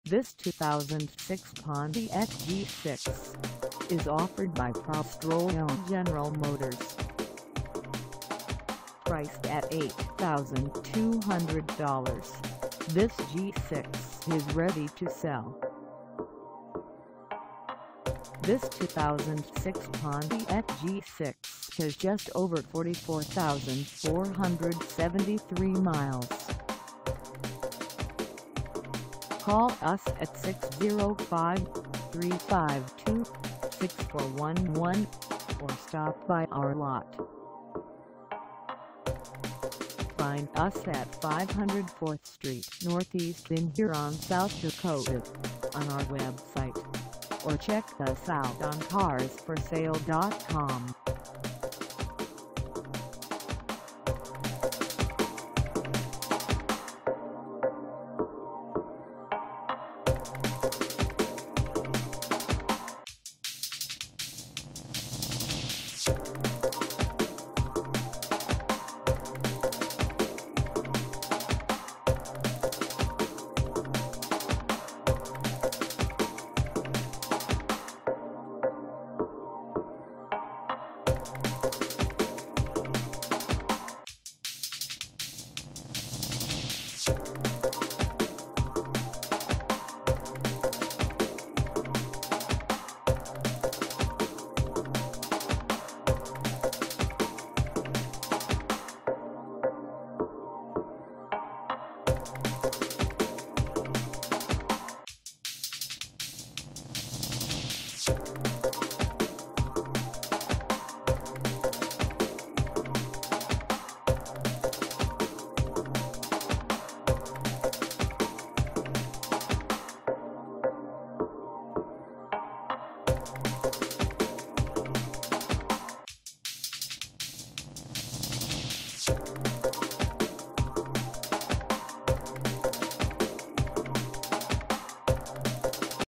2006 Pontiac G6 Base 4dr Sedan for sale in Huron, SD 57350 a
This 2006 Pontiac G6 Base 4dr Sedan is for sale in Huron, SD 57350 at PROSTROLLO GENERAL MOTORS.
Engine: V6 3.5L V6
Transmission: Automatic 4-Speed
Color: SILVER
Mileage: 44473
Contact PROSTROLLO GENERAL MOTORS 500 4th St NE Huron, SD 57350 to test drive this 2006 Pontiac G6 Base 4dr Sedan today.